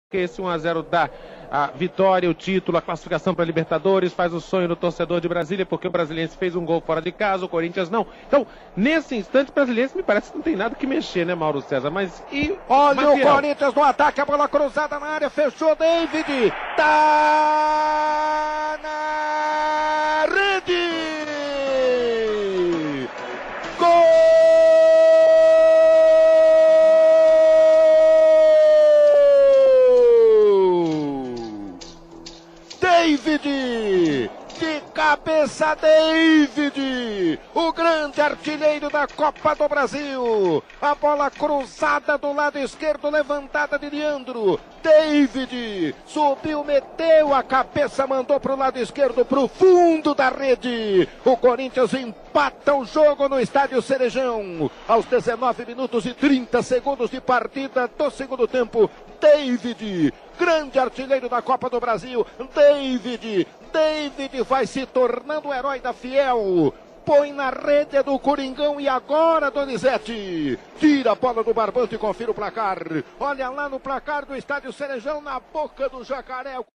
Rádio CBN: Corinthians campeão da Copa do Brasil (15/02/2002)
Gol do empate por 1 x 1 com o Brasiliense na final realizada no Estádio Boca do Jacaré, em Taguatinga (DF), em 15 de maio. Na época, a emissora fazia transmissões nacionais (conjuntas para SP e RJ) com um esquema diferenciado, onde o âncora e comandante de esportes Marco Aurélio debatia as partidas com comentaristas, enquanto vários narradores ficavam em postos nos mais variados jogos para intervir quando saísse gol, rolasse um lance importante ou mesmo para narrar alguns poucos minutos. Neste áudio, Marco chamaria uma participação de um dos comentaristas, Mauro Cezar Pereira, quando Osvaldo Maciel o interrompeu para narrar o gol de Deivid. Na época, a CBN transmitia futebol com os mesmos profissionais da Rádio Globo, exceção aos titulares. Desta forma, no caso específico de SP, enquanto Oscar Ulisses, Paulo Roberto Martins, Romeu César, Ângelo Ananias e Domingos Machado faziam jogos apenas pela Globo AM 1100, os demais narradores, comentaristas e repórteres também transmitiam as partidas pela CBN, tanto alternativas às da Globo quanto as mesmas. À época, os narradores da Globo que transmitiam pela CBN eram Osvaldo Maciel e Ulisses Costa, e o comentarista era Luiz Augusto Maltoni.